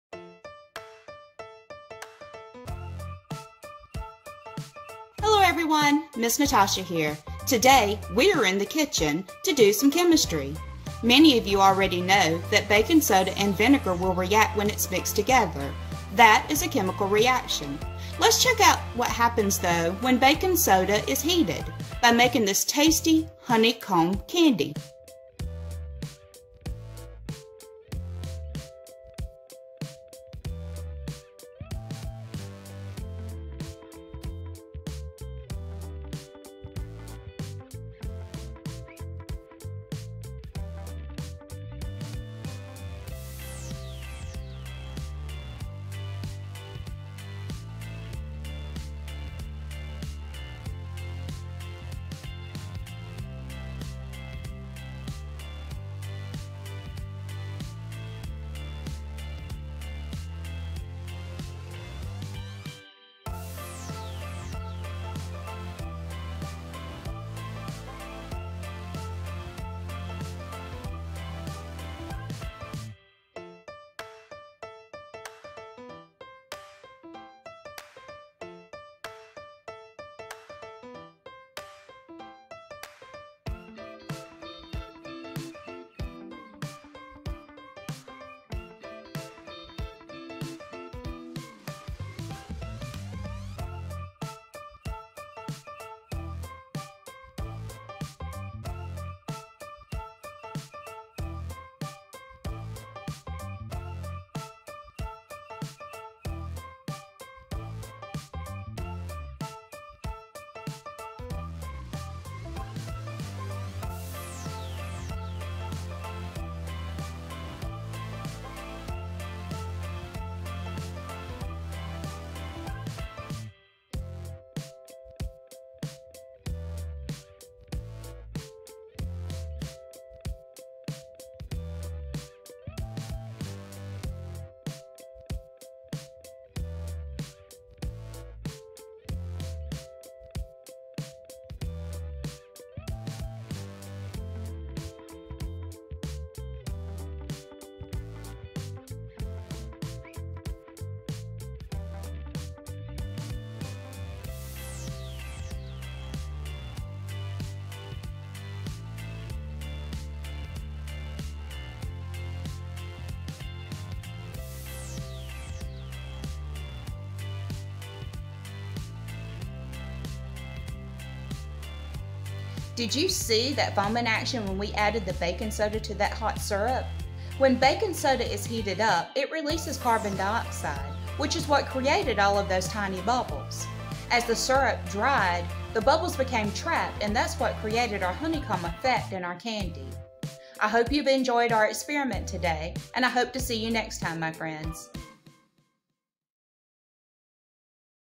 Chemistry in the Kitchen
Come make a Honeycomb Candy with Mrs. Natasha!